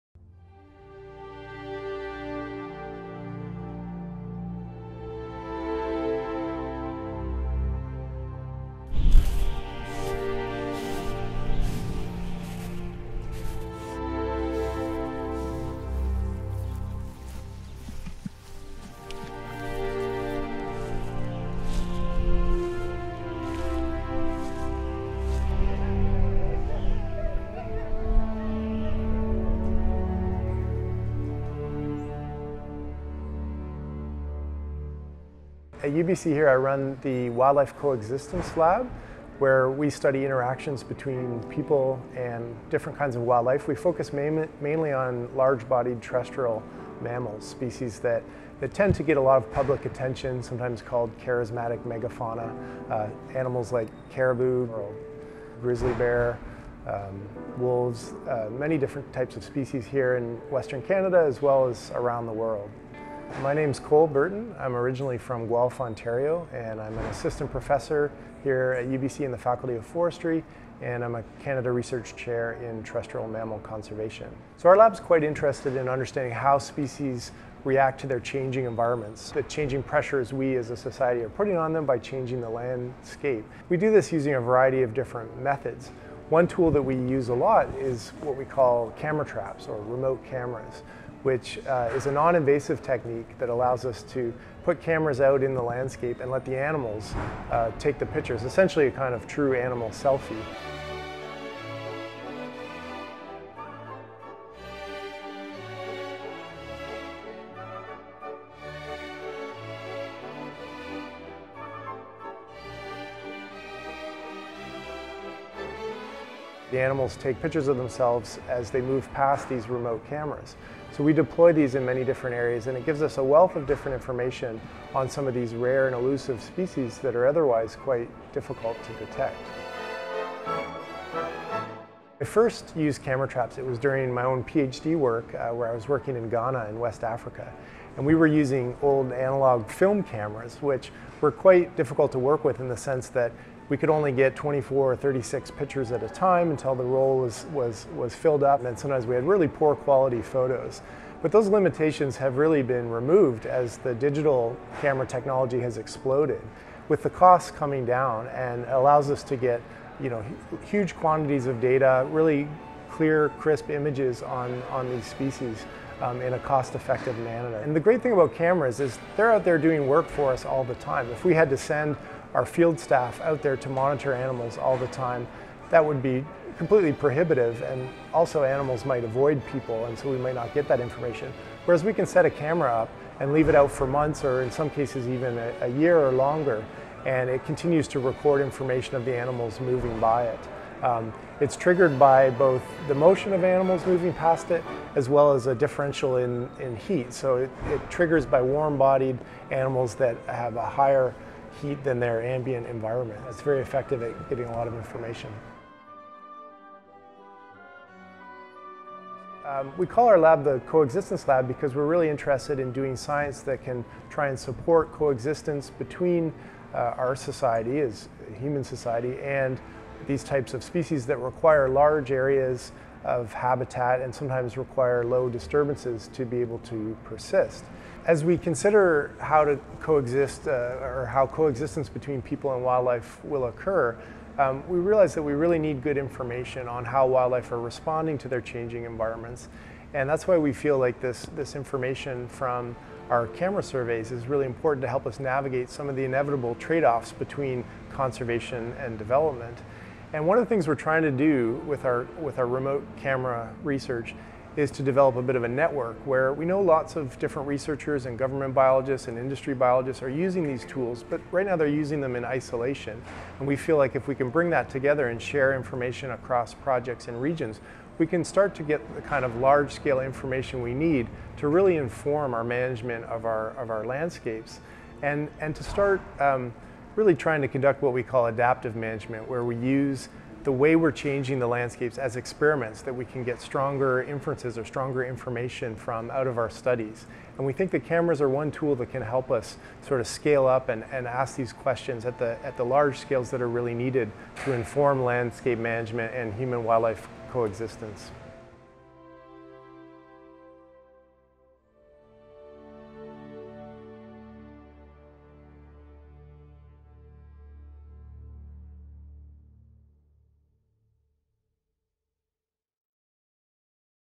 Eyes in the Wild - Researchers Revealed
Researchers Revealed is an exciting new series profiling the research performed in UBC's Biodiversity Research Centre. Produced by Philippe Roberge, the series looks at new ways in which researchers harness technology to study the natural world in ways that were not possible before.